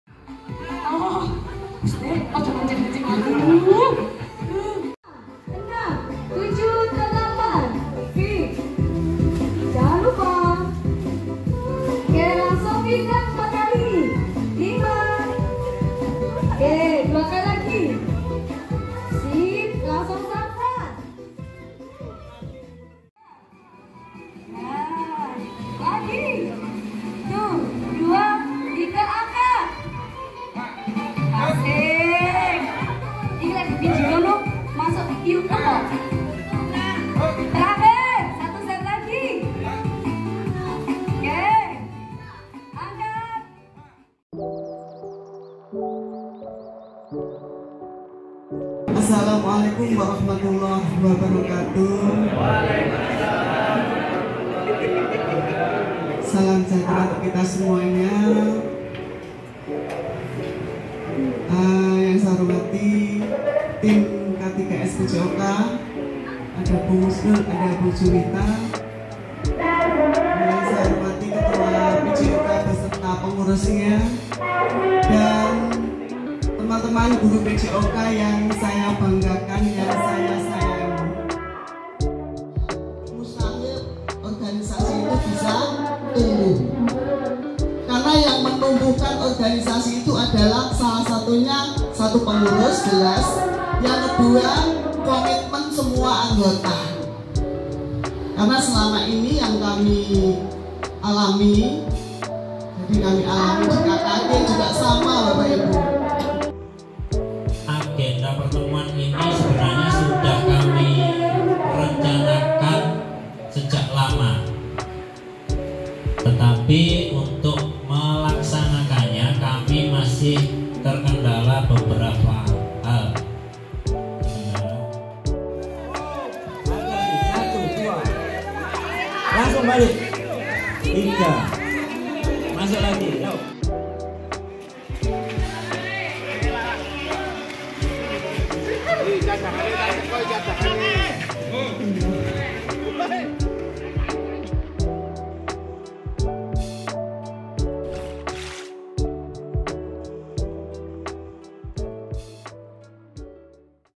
KKG PJOK KEC LOWOKWARU RAPAT SOSIALISASI PROGRAM KERJA DI SD MUHAMMADIYAH 4 KOTA MALANG
pada pertemuan kali ini guru-guru pjok kec lowokwaru melakukan rapat sosialisasi program kerja yang akan di susun, dan paginya melakukan senam bersama, melakukan permainan tradisional terompah, gobak sodor dengan antusias untuk menjalin silaturrahmi antar guru pjok lowokwaru.